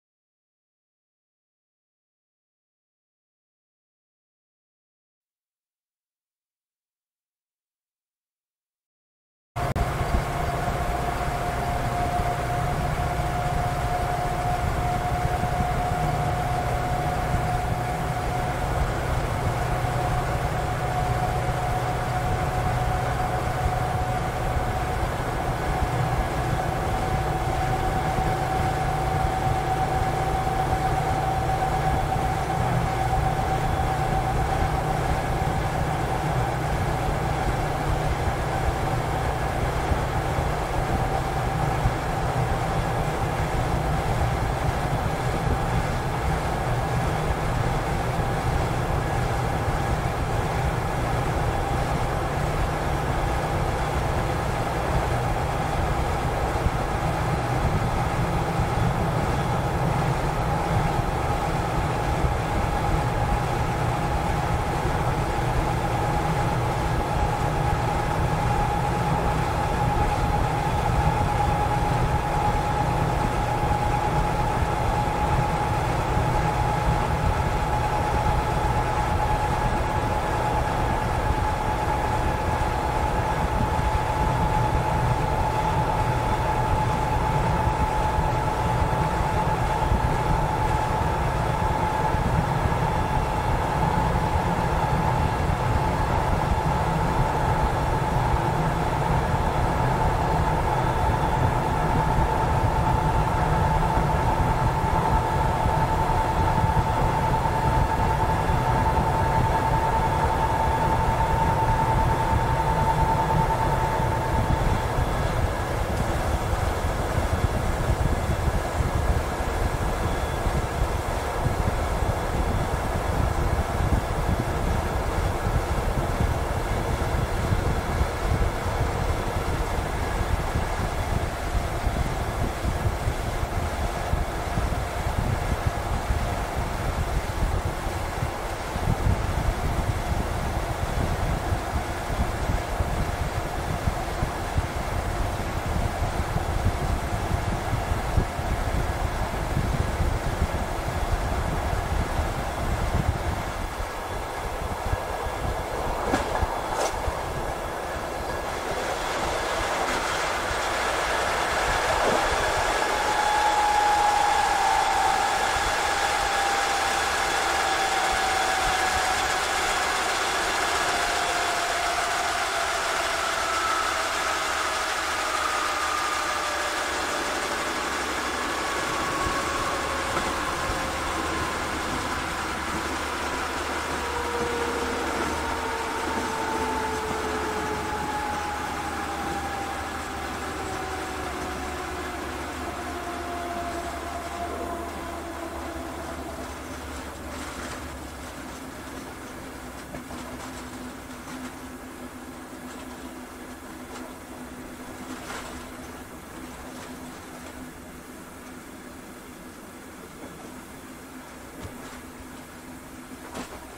🔴 LIVE TRAIN 24/7 🔴 WINTER CAB VIEW
[LIVE] 🔴🌟 Winter Cab View from  24/7 🔴🌟 two of the most SCENIC RAILWAYS in the WORLD

LIVE TRAIN 24/7 & LIVE CHAT. Virtual travel experience. See the beautiful landscapes of winter Norway from the NSB Di4 Locomotive Norwegian State Railways (NSB). Beautiful Winter Norwegian Railway! Live Stream Train Cab Ride by Nordland Line from Trondheim to Bodø. Norway in a nutshell.